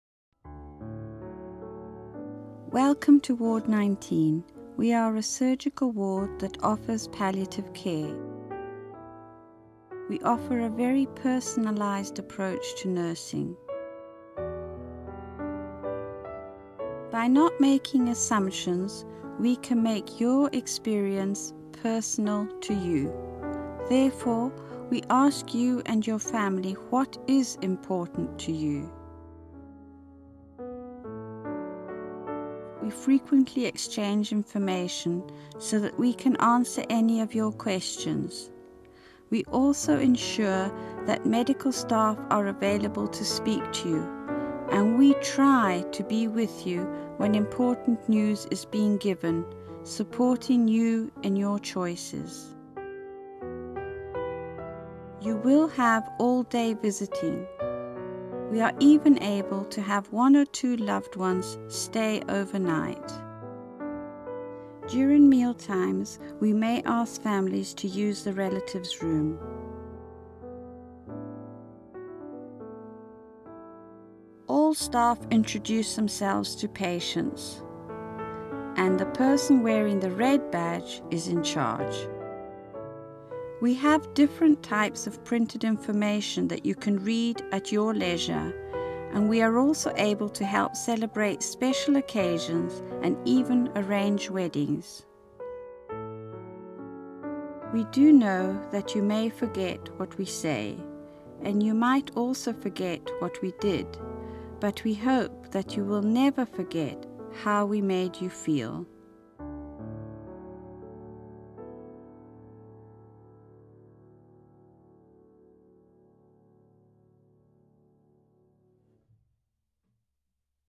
NHSGGC - Royal Alexandra Hospital, Ward 19 - Palliative Care
An overview of the palliative care services provided in Ward 19 of the Royal Alexandra Hospital in Paisley.